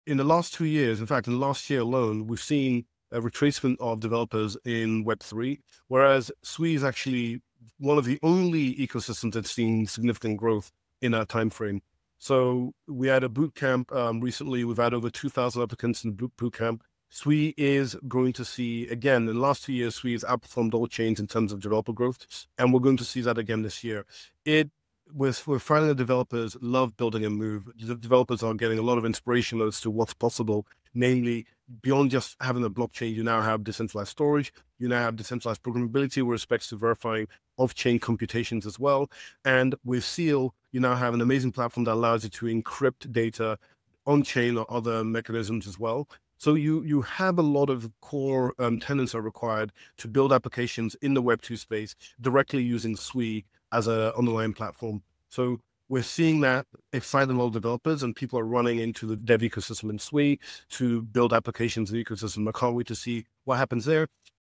Why Devs Are Flocking to Build on Sui
Despite a Web3 slowdown, Sui's dev ecosystem is booming—powered by Move and new core primitives.